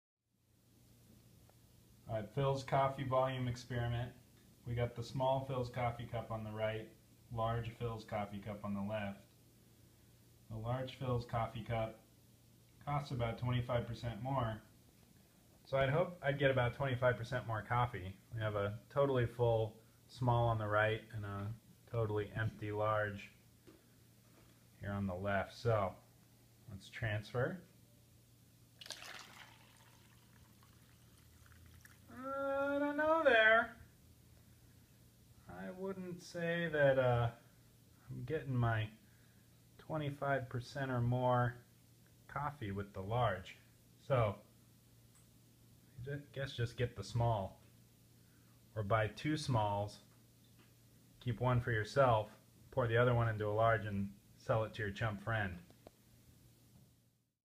Large vs. small Philz coffee cup coffee transfer
Check out the difference in volume between a large ($4) and small ($3) coffee from Philz.